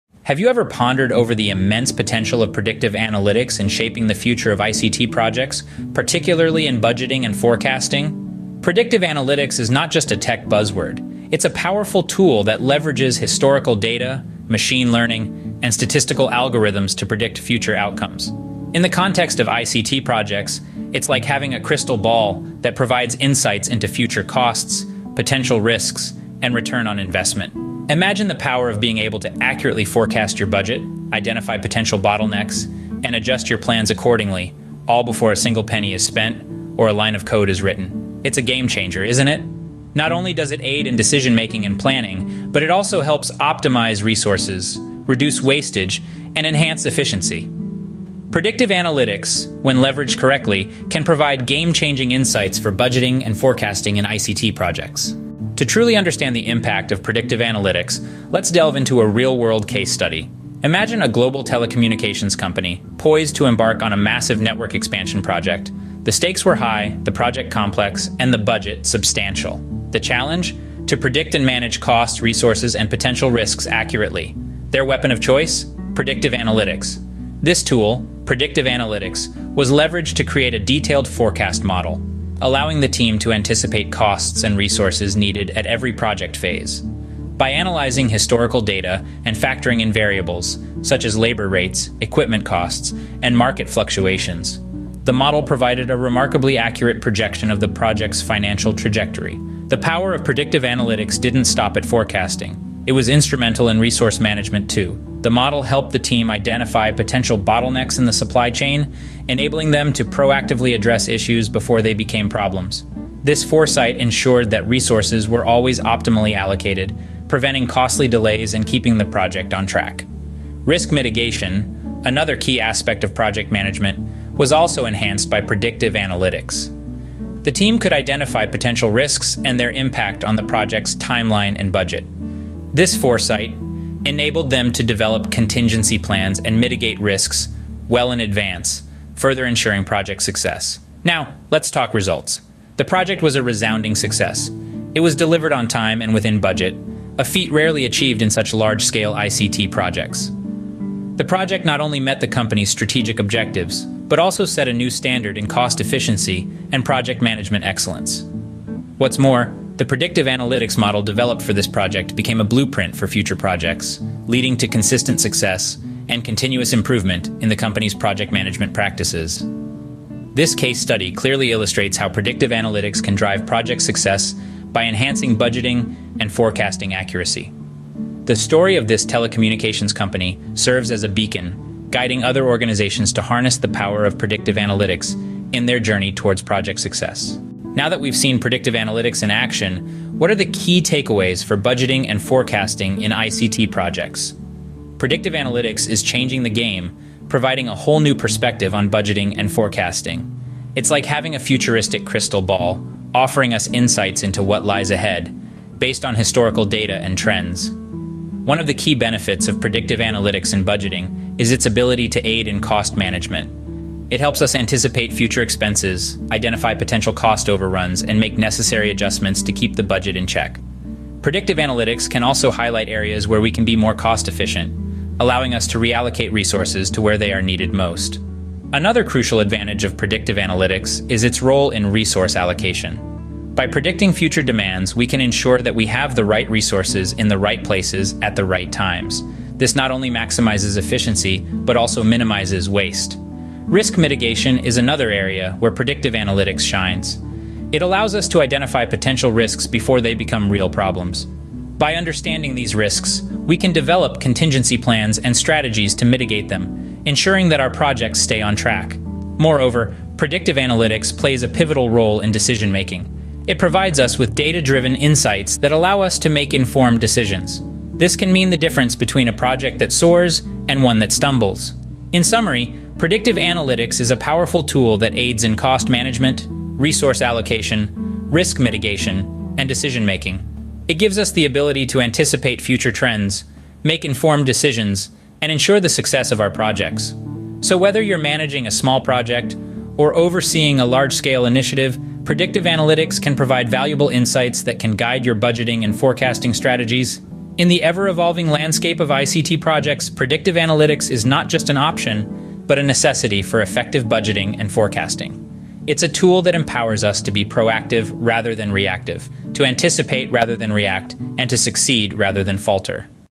What is Predictive Analytics?
In today's video, we're diving deep into Predictive Analytics, uncovering everything you need to know to stay informed and ahead of the curve.

Predictive analytics in project management involves using historical data, statistical algorithms, and machine learning techniques to identify the likelihood of future outcomes based on past project performance. This approach helps project managers anticipate potential risks, make informed decisions, and optimize project planning and execution for better results.

OUTLINE:

00:00:00 The Power of Predictive Analytics in ICT Projects
00:00:55 A Success Story in ICT
00:03:07 Predictive Analytics in Budgeting and Forecasting